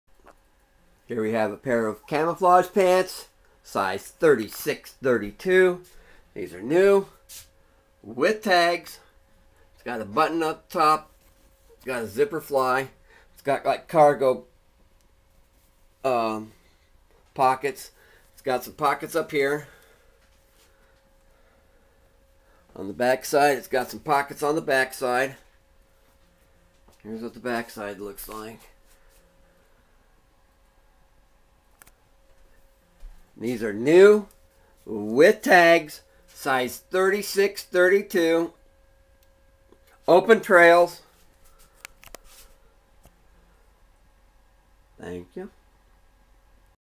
Open Trails Camouflage Pants New With Tags 36 X 32
For sale a NEW with tags camouflage pants. Pants have 6 pockets (2 regular pockets, 2 cargo pockets on the legs with buttons, and 2 back pockets with buttons. Front has a button waist and zipper fly. This item is for sale.
BRAND : Open Trails
SIZE : 36 X 32
COLOR : Camouflage
WAIST : 36 inches (91.44 cm)
INSEAM : 32 inches (81.28 cm)
OUTER SEAM : 42 inches (106.68 cm)
MATERIAL : 100% cotton
STYLE # :
RN # : 67227
CLEANING : Machine Wash Cold